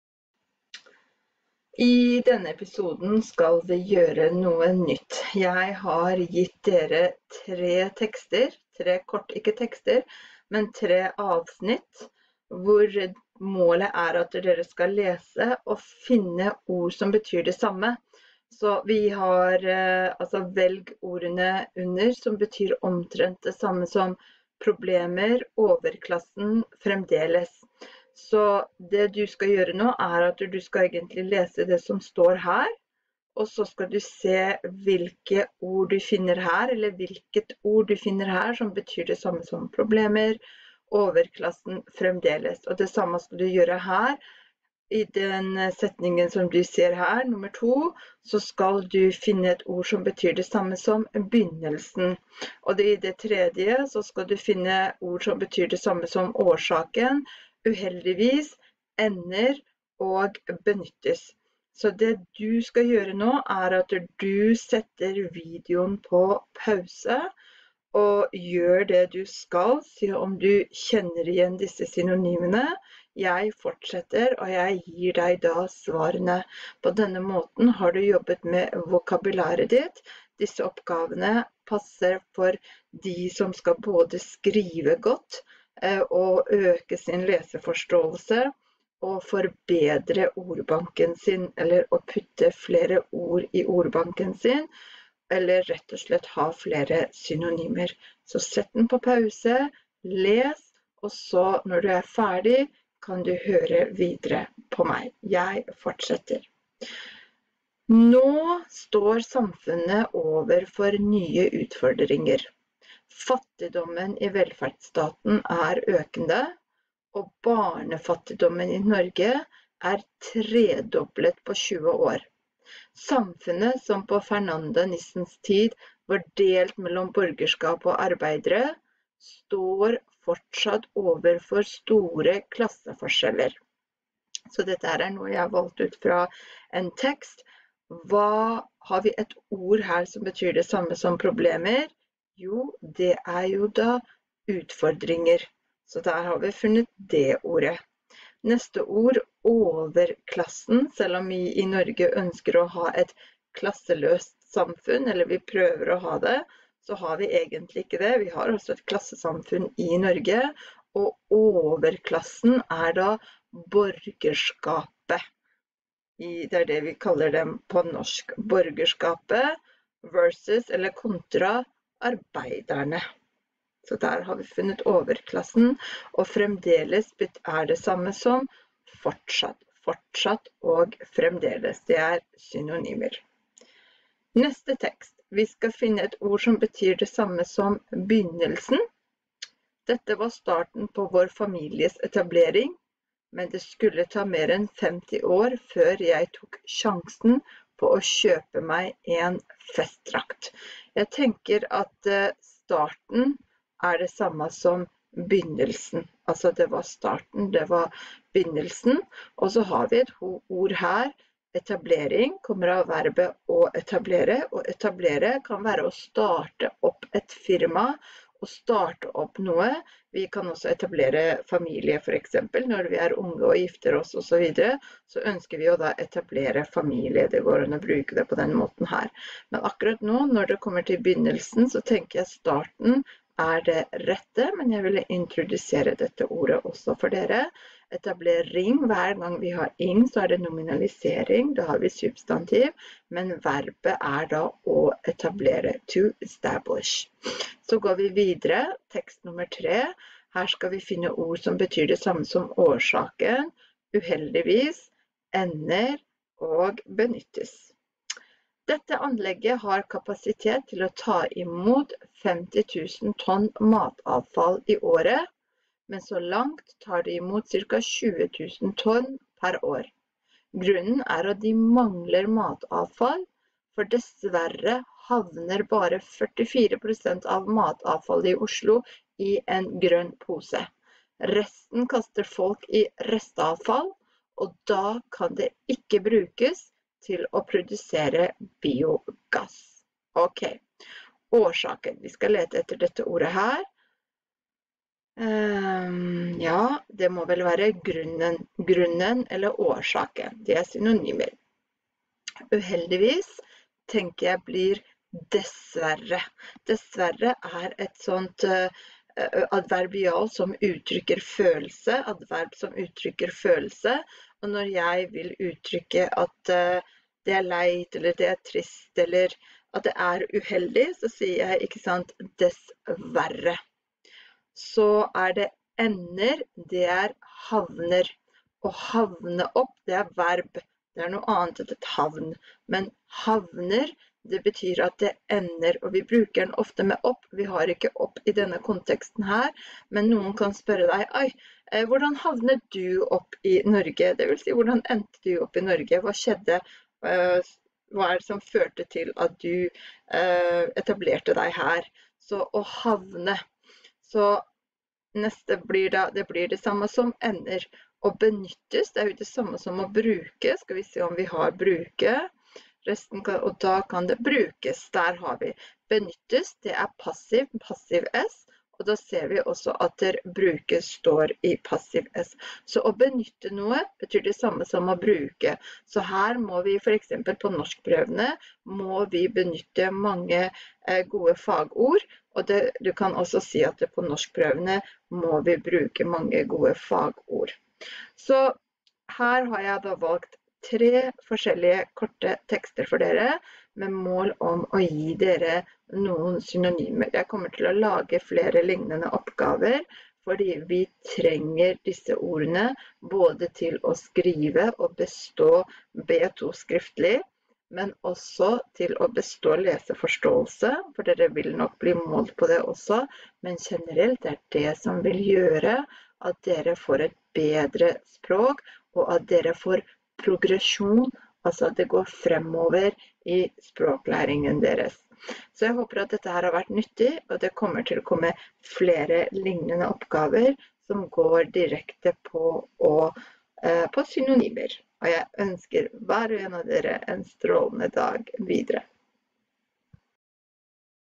Video 140 - Synonymer 1
I denne videoen gir jeg dere 3 korte tekster, målet er å finne diverse synonymer i tekstene. Denne øvelsen forbedrer vokabularet deres og vil hjelpe dere med å bestå deler av leseforståelsesdelen i norskprøvene.